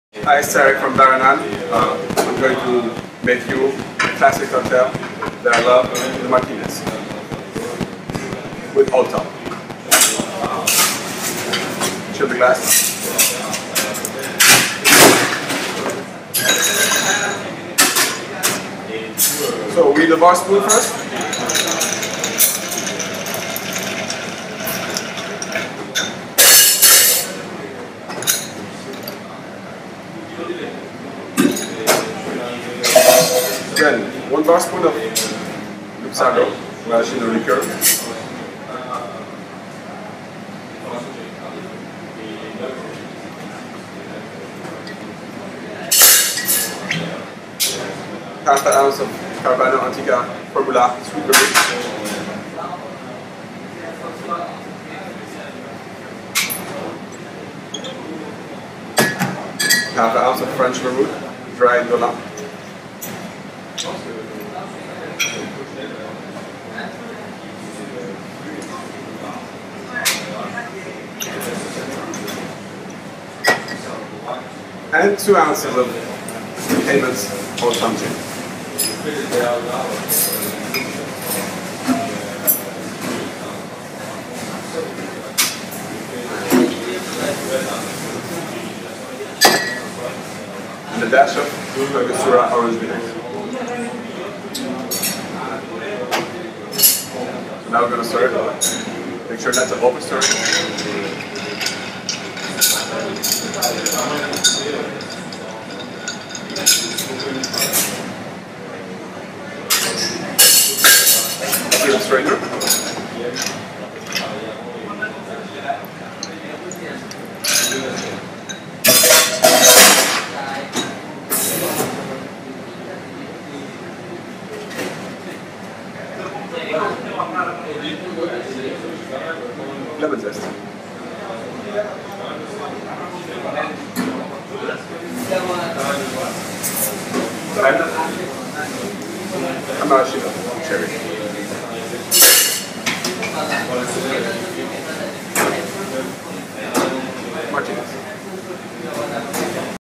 Tarek Nini from Le Ballroom (Paris) : ​ Martinez Old Tom
Tarek Nini from le Ballroom makes for us his twist on a Martinez called​ Martinez Old Tom